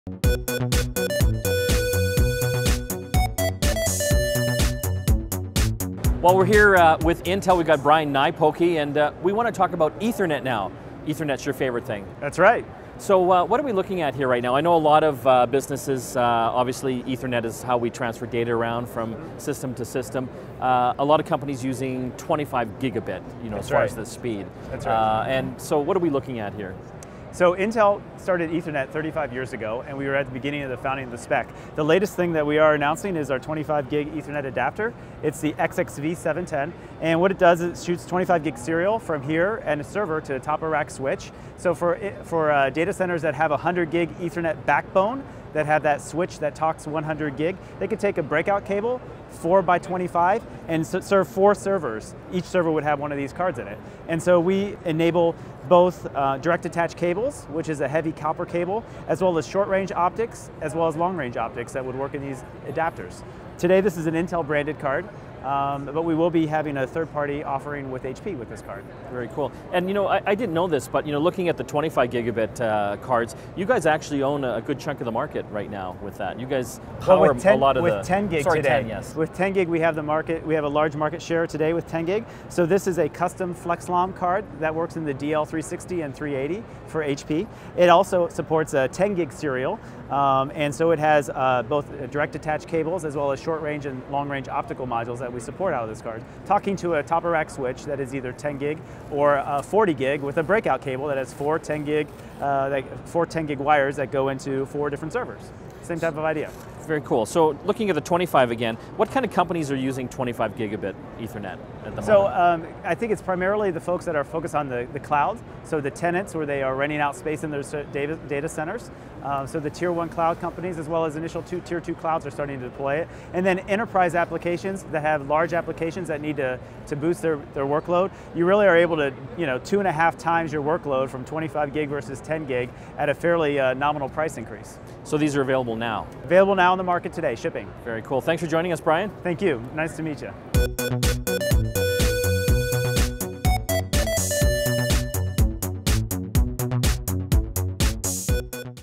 25Gb and 10Gb Ethernet adapters
Mike talks to Brian Niepoky from Intel about ethernet adapters for servers and cloud storage.